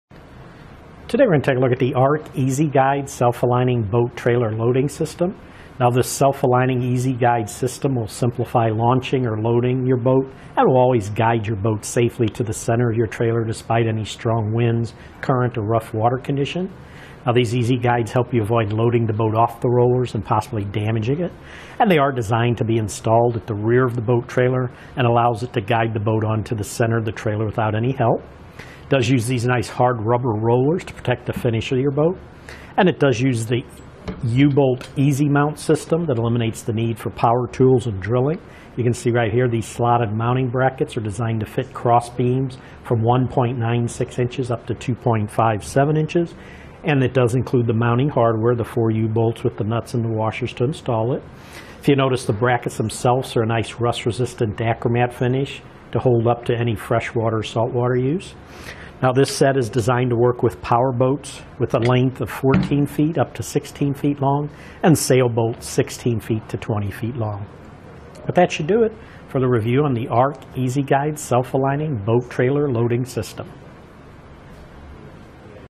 etrailer | Review of Ark Boat Trailer Parts - Guides - AR64FR
Today we're going to take a look at the Ark Easy Guide, self aligning boat trailer loading system. Now this self aligning easy guide system will simplify launching or loading your boat. And it'll always guide your boat safely to the center of your trailer, despite any strong winds, current or rough water condition. Now these easy guides help you avoid loading the boat off the rowers, and possibly damaging it. And they are designed to be installed at the rear of the boat trailer, and allows us to guide the boat onto the center of the trailer without any help. Does use these nice hard rubber rollers to protect the finish of your boat, and it does use the U-bolt easy mount system that eliminates the need for power tools and drilling.You can see right here, these slotted mounting brackets are designed to fit cross beams from 1.96 inches up to 2.57 inches.

And it does include the mounting hardware, the four U-bolts with the nuts and washers to install it. If you notice, the brackets themselves are a nice rust resistant dachre matte finish to hold up to any freshwater saltwater use. Now this set is designed to work with powerboats, with a length of 14 feet up to 16 feet long, and sailboats 16 feet to 20 feet long. But that should do it for the review on the Ark Easy Guide, self aligning boat trailer loading system.